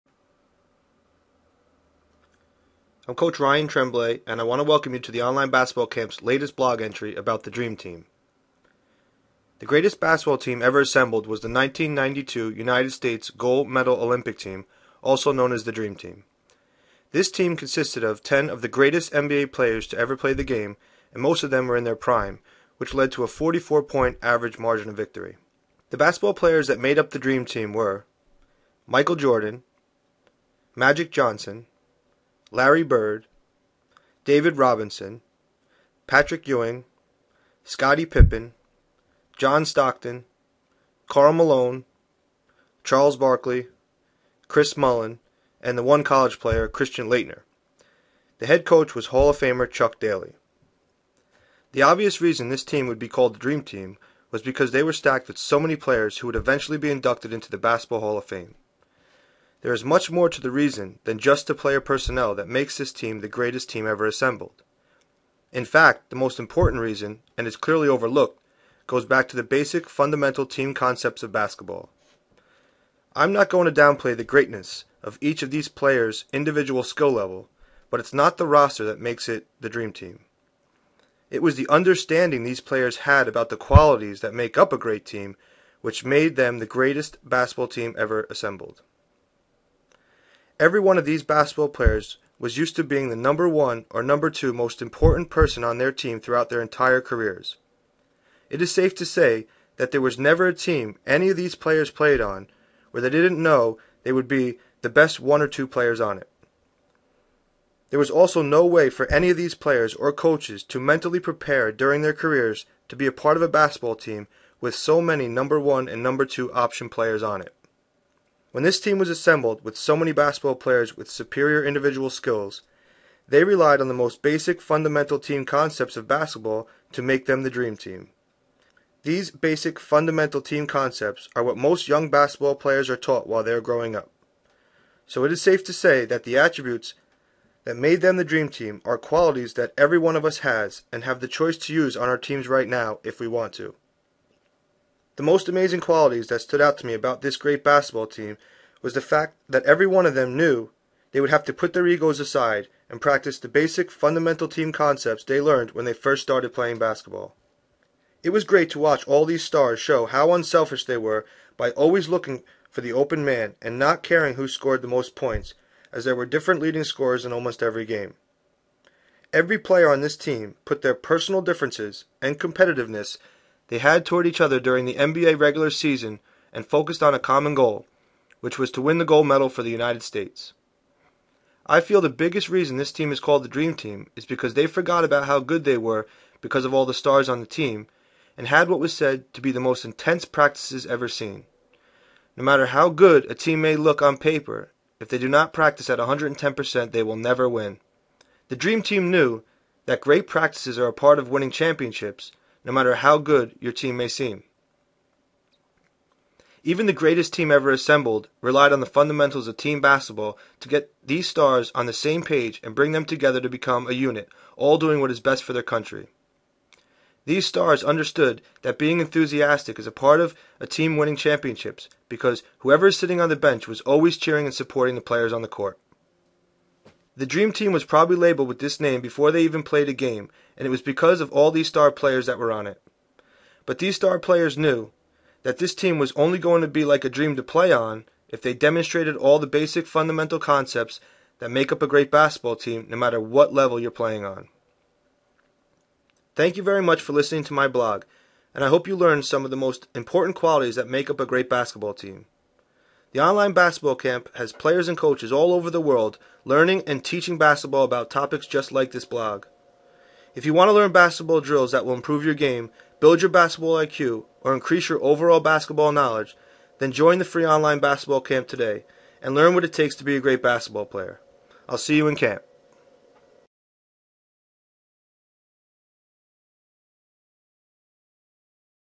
Dream Team | The Greatest Basketball Team Ever
The Dream Team was the greatest basketball team that was ever assembled and they won the 1992 Olympipc Gold Medal in Barcelona, Spain. The players on the team were: Michael Jordan, Magic Johnson, Larry Bird, David Robinson, Patrick Ewing, Scottie Pippen, John Stockton, Karl Malone, Charles Barkley, Chris Mullin & Christian Laetner. The head coach was Hall of Famer Chuck Daley.
This video talks about the reasons it was the greatest team ever assembled and labeled he Dream Team.

You can learn hundreds of basketball drills & tips in the Online Basketball Camp where you can join for free